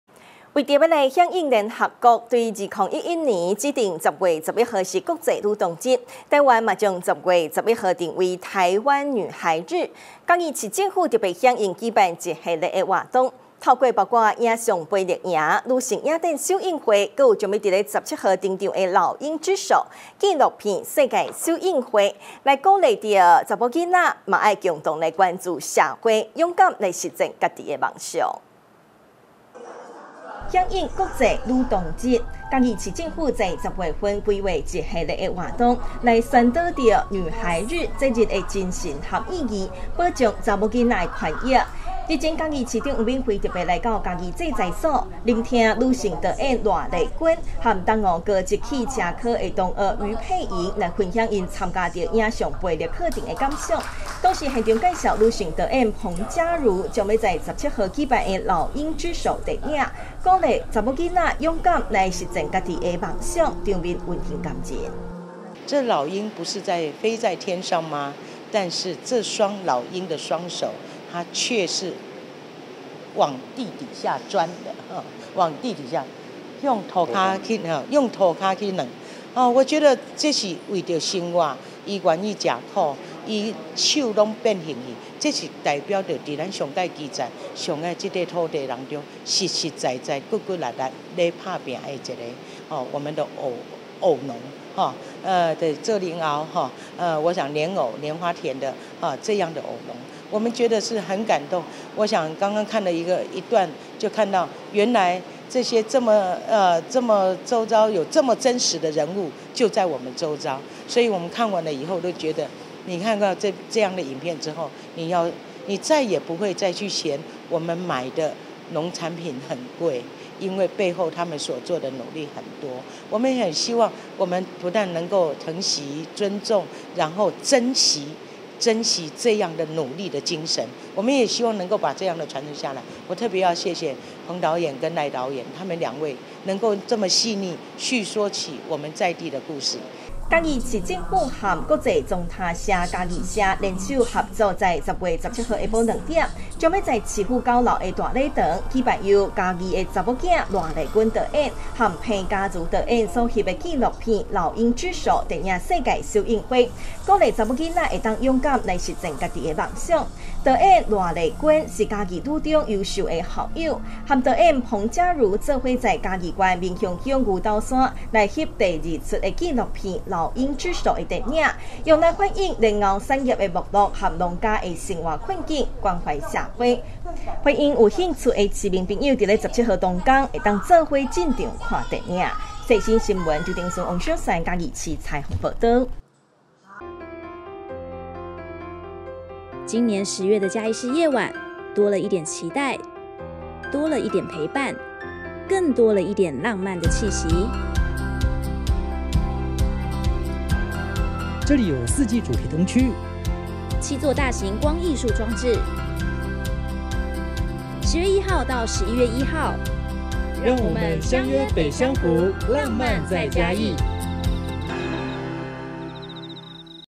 世新新聞 女力拍片紀錄家鄉產業 老鷹之手17日首映
109年10月12日 (一) 世新新聞

女力拍片紀錄家鄉產業 老鷹之手17日首映

響應聯合國自2011年指定10月11日為「國際女童日」，台灣將10月11日定為「臺灣女孩日」，嘉義巿政府響應舉辦系列活動，透過影像培力營、女性影展首映會以及將在17日登場的「老鷹之手」紀錄片世界首映會，鼓勵女孩走出課堂，關注社會，勇敢實踐自已的夢想。